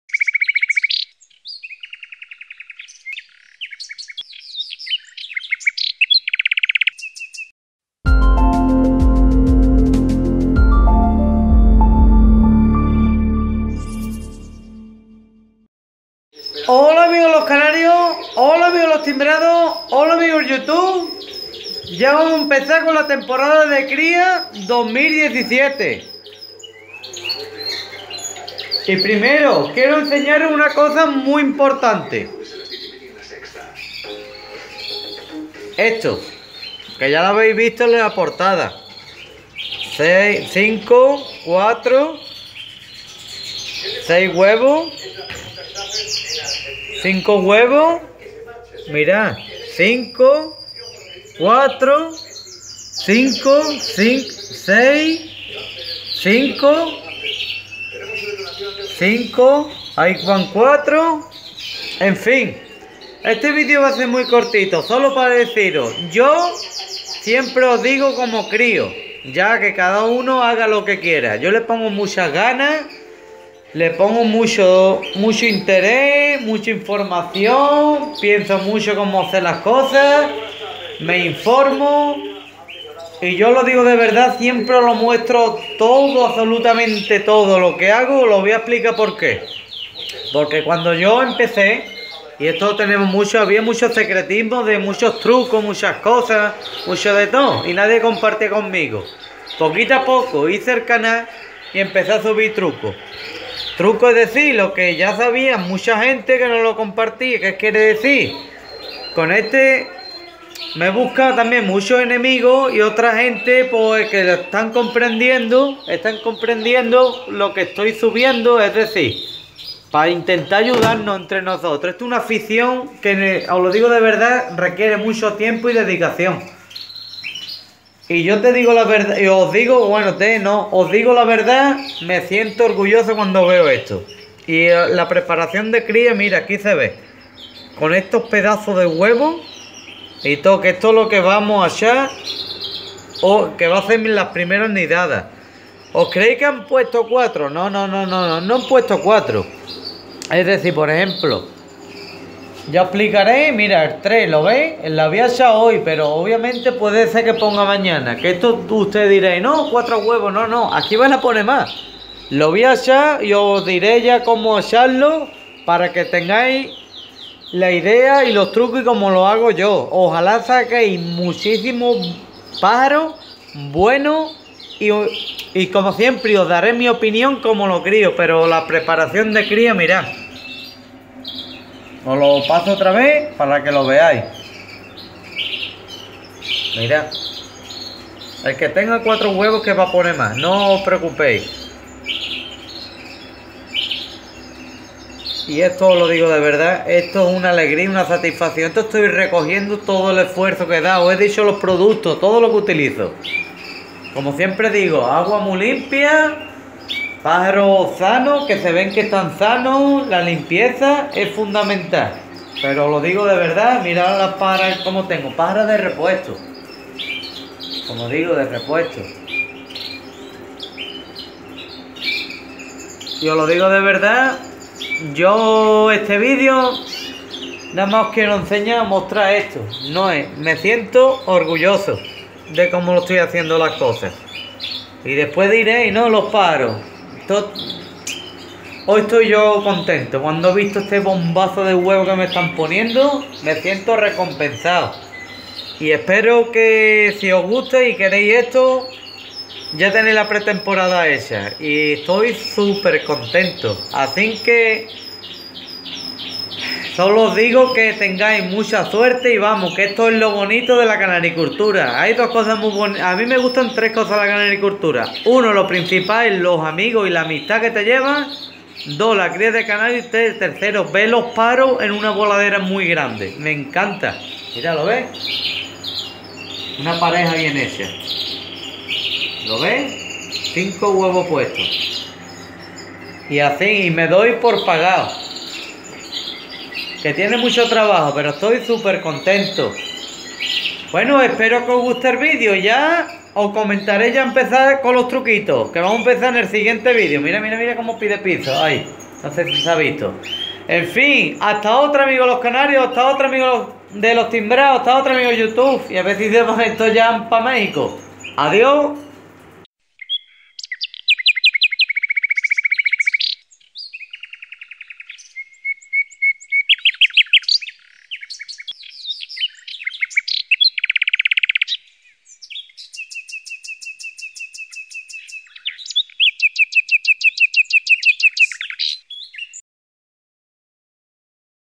UN BUEN TRABAJO
SI QUEREIS LOS PRODCUTOS PARA LA PREPARACION LO PODEIS CONSEGUIR
OS MUESTRO LA PREPARACION DE CRIA 2016 Y SUS CONSECUENCIAS QUE SON 5 HUEVOS DE MEDIA EN EL AVIARIO ESPERO QUE USTEDES TENGAIS IGUAL O MAS QUE YO